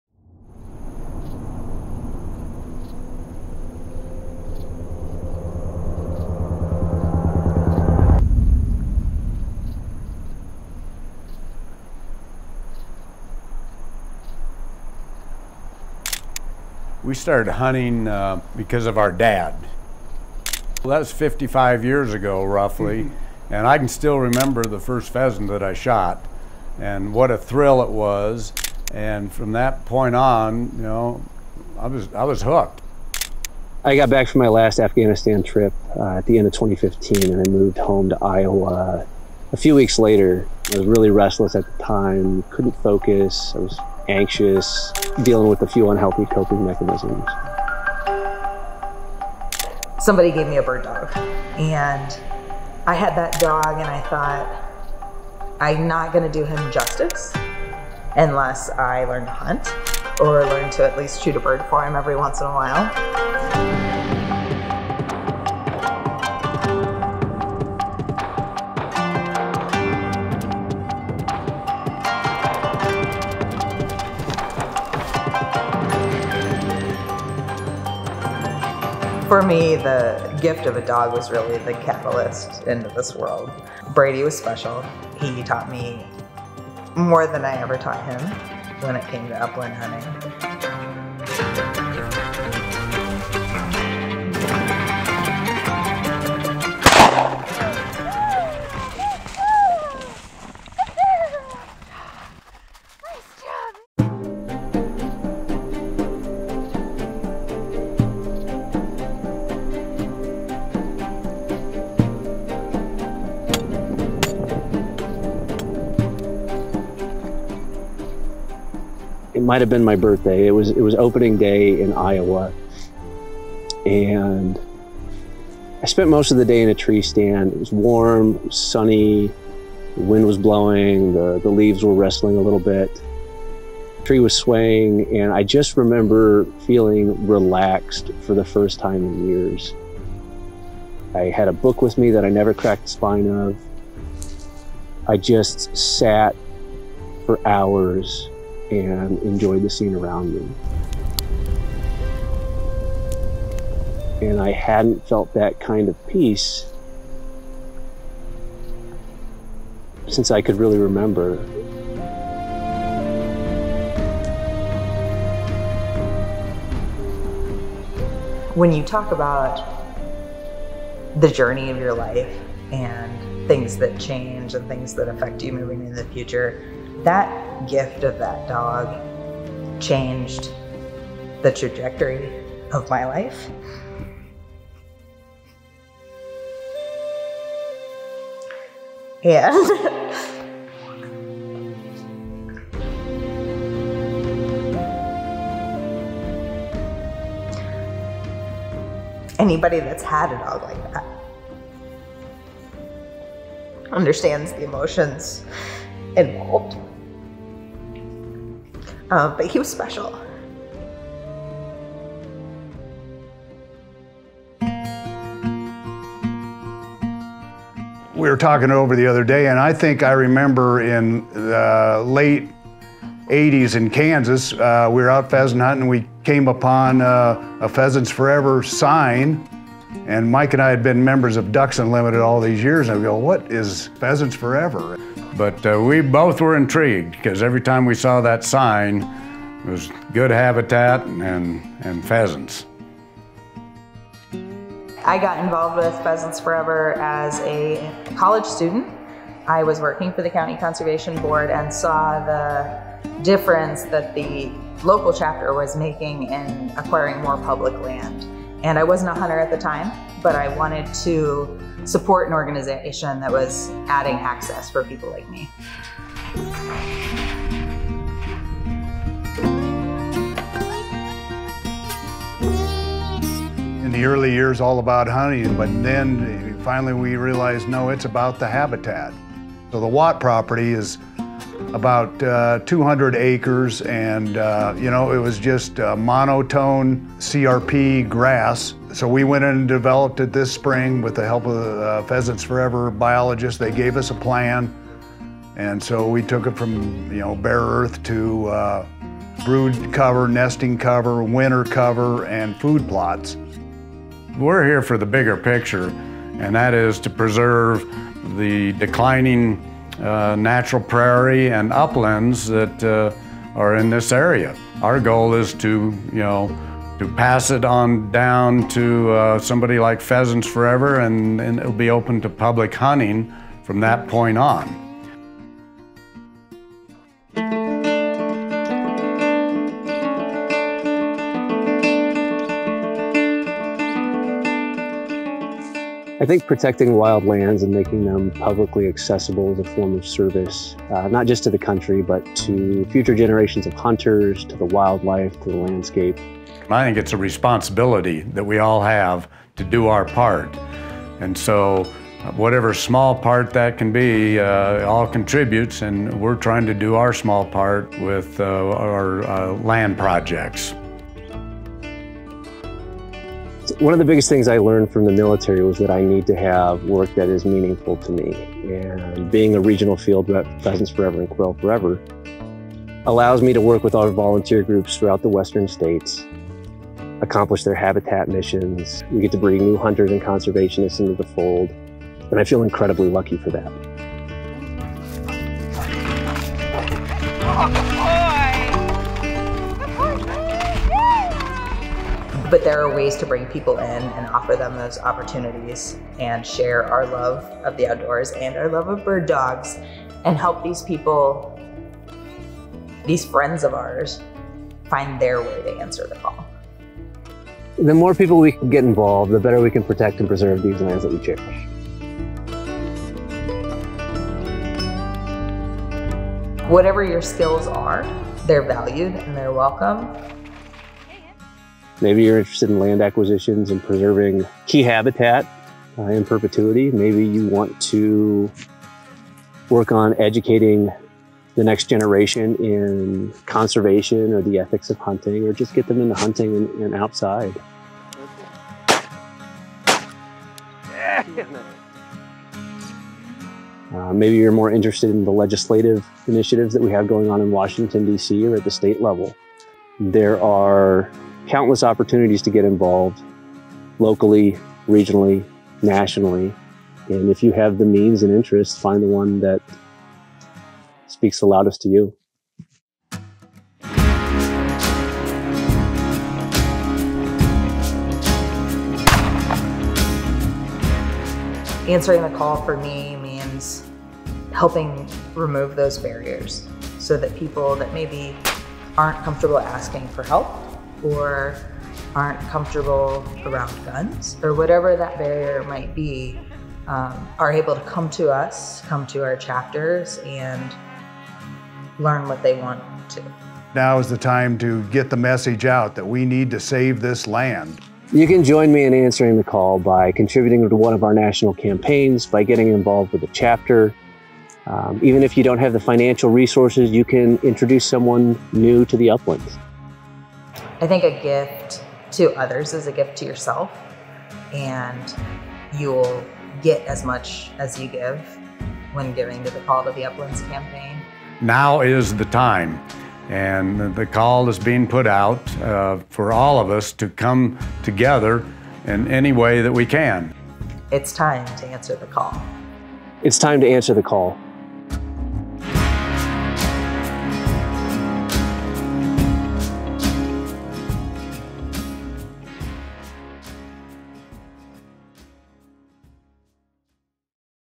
Pheasants Forever & Quail Forever Present: Answer the Call
There comes a time in all our lives when a spark is lit. Passivity transforms into action, newfound passion creates untamable energy, and the realization you can make a difference for something you care deeply about turns into tangible results.

Follow along with the journey of Rose, David, and the Fitz brothers as they reveal their own personal motivations for supporting Pheasants Forever and Quail Forever and see how their unique stories all intersect at a sense of purpose and commitment to Answer the Call.

For more information on how you can help answer the Call of the Uplands, please